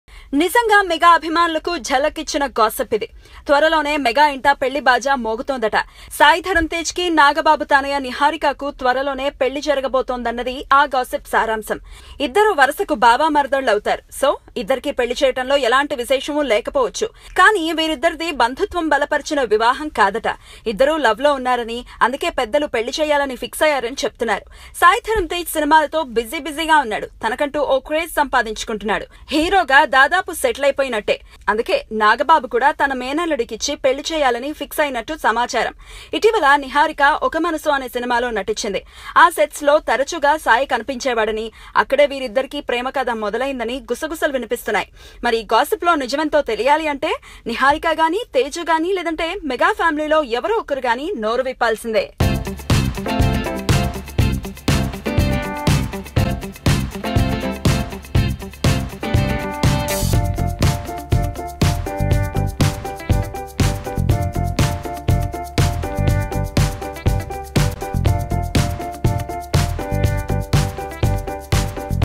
Sai Dharam Tej To Marry Niharika ?
Buzz is that, Sai Dharam Tej Will Marry Niharika Konidela, Daughter of Naga Babu. Watch This Video To Know Whether This News is True or Not.